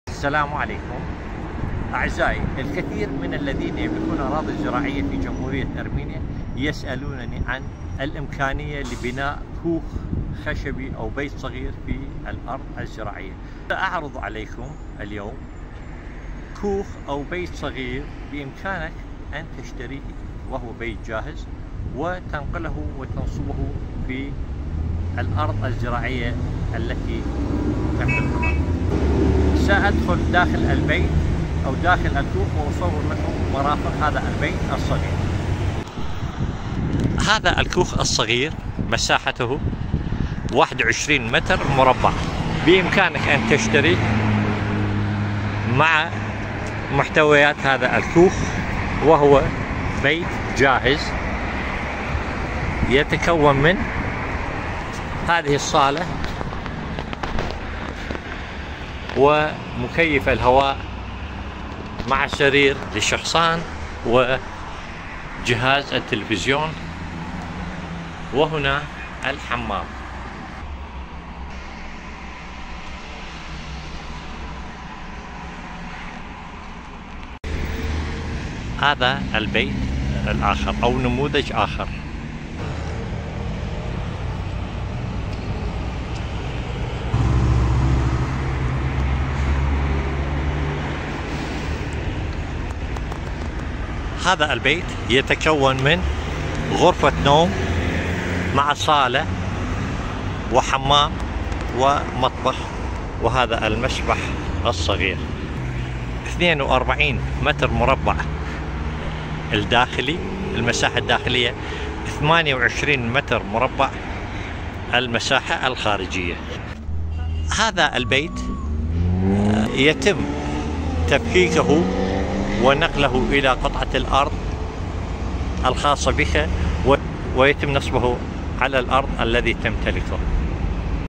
بيوت جاهزة للمزارع في ارمينيا
في هذا الفيديو، نستعرض لكم البيوت الجاهزة للمزارع في أرمينيا، التي تتميز بتصميماتها العصرية وسرعة تركيبها، مما يجعلها خيارًا مثاليًا للراغبين في الإقامة الريفية أو الاستثمار. بفضل سعرها المميز، حيث تبدأ التكلفة من 500 دولار للمتر المربع فقط، توفر هذه البيوت حلاً اقتصاديًا للعيش في قلب الطبيعة بدون الحاجة لتكاليف بناء ضخمة أو أوقات انتظار طويلة.

سنأخذكم في جولة لمشاهدة مراحل التركيب السريعة لهذه المنازل، والتي يمكن أن تكون جاهزة في فترة زمنية قصيرة جدًا، بالإضافة إلى تفاصيل التصميم الداخلي والخارجي التي تجعل من هذه البيوت مكانًا عمليًا ومريحًا في آنٍ واحد. تابعوا الفيديو لتتعرفوا على كافة التفاصيل التي تهمكم للاستثمار أو السكن في أرمينيا!